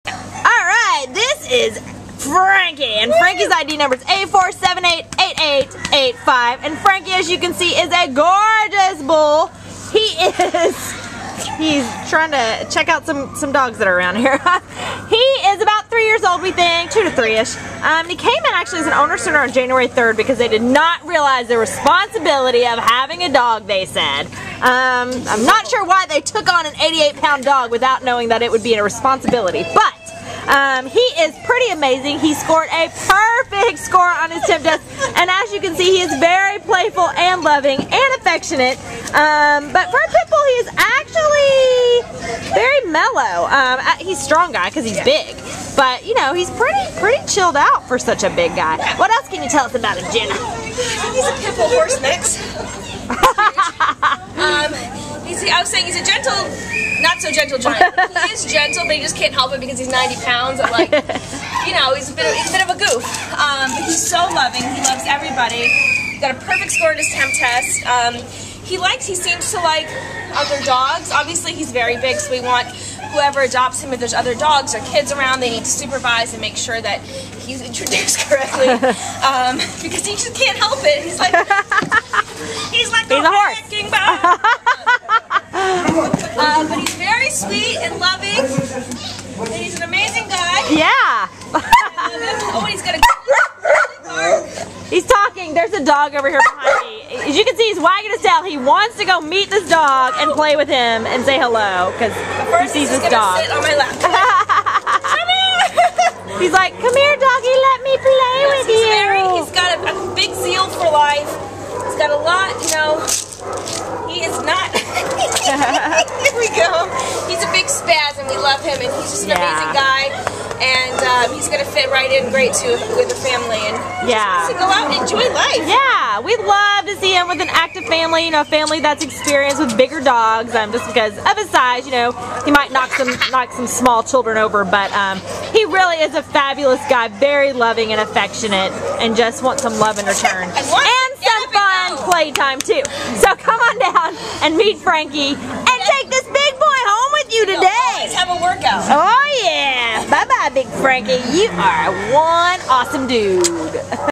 A4788885 Franky
A4788885 Franky is a friendly 3-year-old gray-and-brown male American Staffordshire Bull Terrier mix who arrived at the Baldwin Park Animal Care Center on January 3rd because his owner could no longer care for a pet. Weighing 88 lbs, Franky passed his shelter-administered temperament test with a perfect “A.” What we love so much about Franky is that he is equally playful and affectionate. With his large build, he is certainly a strong and sporty dog who is perfect for an active family eager to both exercise with their new best buddy and bring him along on all of life’s adventures. But he is also just so sweet and mellow, and he loves everyone indiscriminately! Franky also greatly enjoys the company of other dogs; he wags his tail happily when he spots another pup, and he greets said pup eagerly to play. Because of this enthusiasm, he should be properly introduced to other dogs. Franky is both a fun-loving guy and a “gentle giant” who will make a magnificent indoor pet.